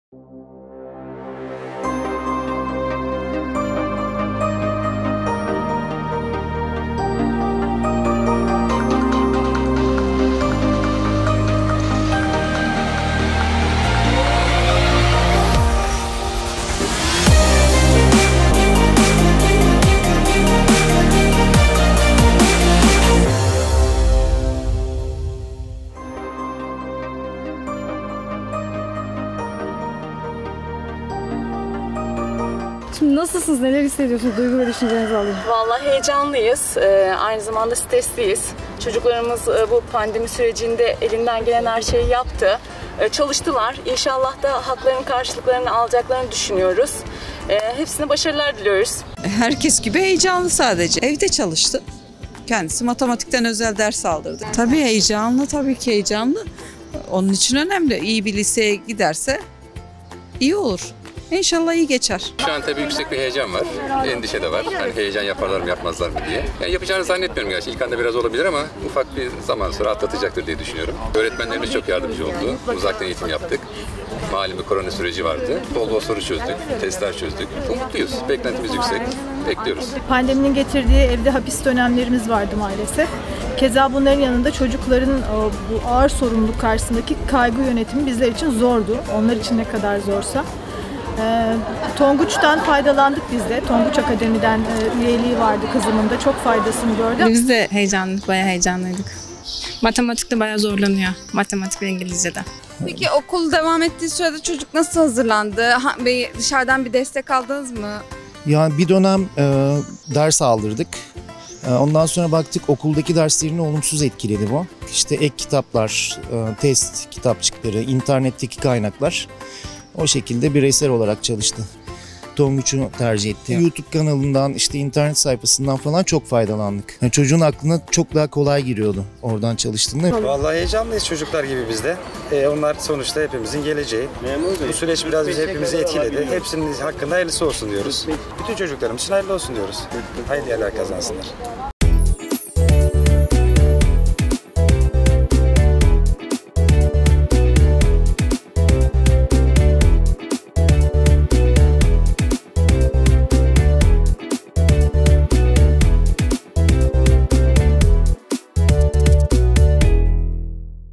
2020 LGS için Velilerin Düşünceleri Neler?
2020 LGS ile ilgili velilerimizin düşünleri neler? Sokak röportajımızda sorduk. 😊